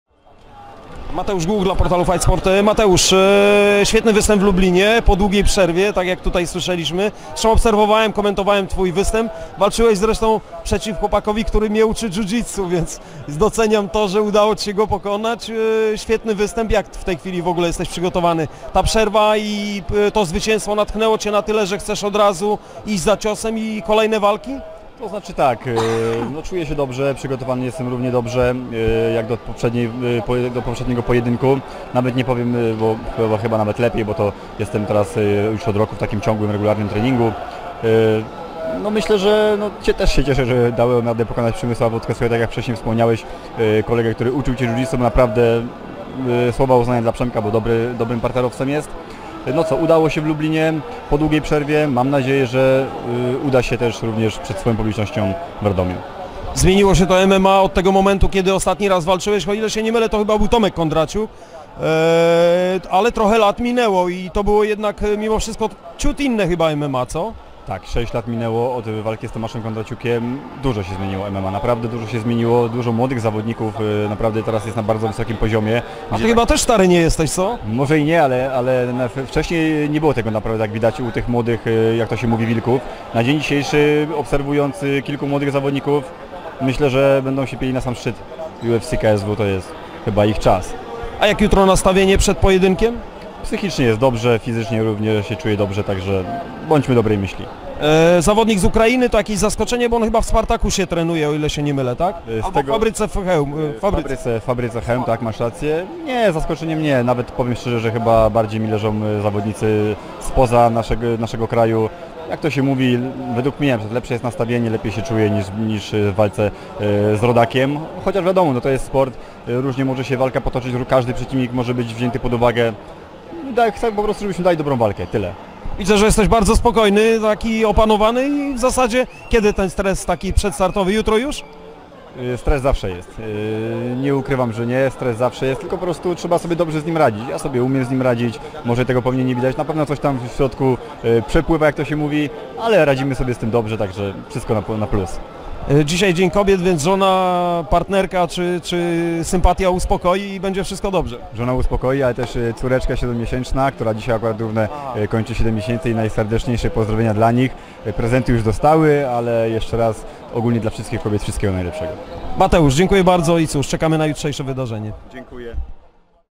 Mateusz Głuch przed TFL 16: Stres zawsze jest! Trzeba umieć sobie z nim radzić!
Mateusz Głuch przed walką z Vitalyem Dżusem na gali TFL 16 ''Radom vs Reszta Świata'', która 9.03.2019 odbędzie się w Radomiu